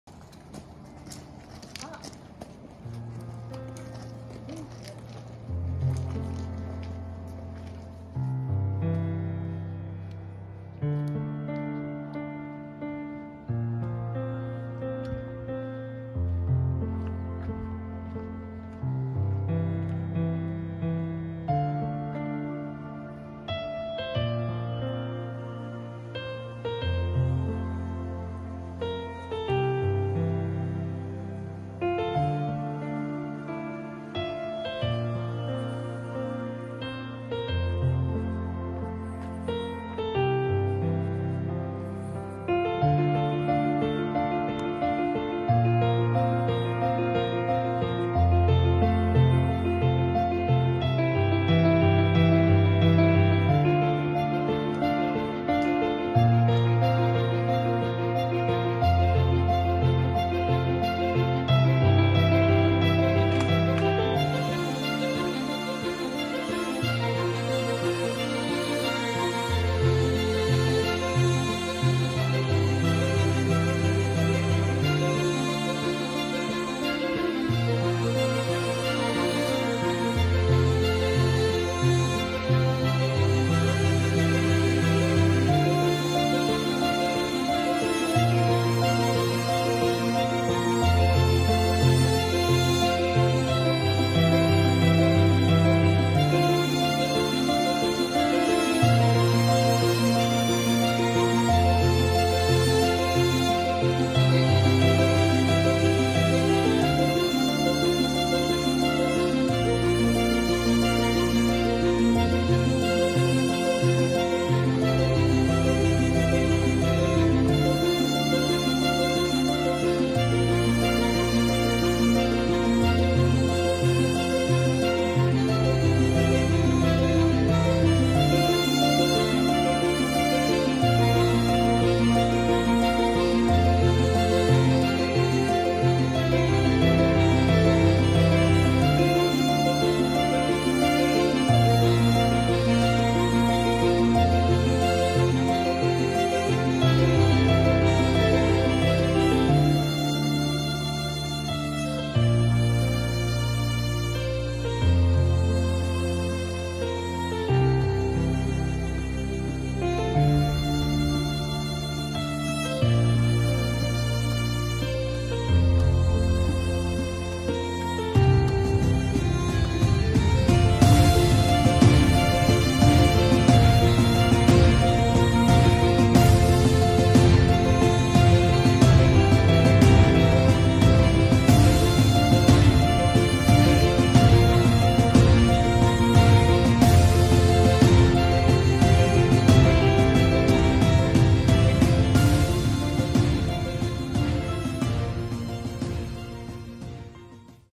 【長野県の名刹】光前寺もみじまつりライトアップ
長野県駒ヶ根市の名刹「光前寺」の紅葉ライトアップに来ました。今日が初日でした。
まだ紅葉には早かったです（笑）
まだまだライトアップは続きますので、ぜひタイミングを見て行かれてはいかがでしょうか。